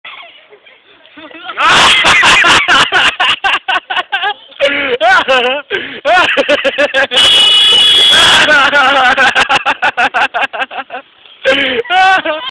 Loic un putain de gars !!!Lol !!!
Un gars un peu foufou qui décide de jouer un scuicidaire ...Voilà ce que ça donne !!!De plus ...Le chauffeur s'est montré très généreux !!!Il a meme trop bien joué son role !!!Mdr !!!Sacré Lolo !!!Heureusement qu'il en existe des gars comme lui ...Sinon je peux vous dire qu'on se ferait bien chier .On crèverait tous d'ennuis .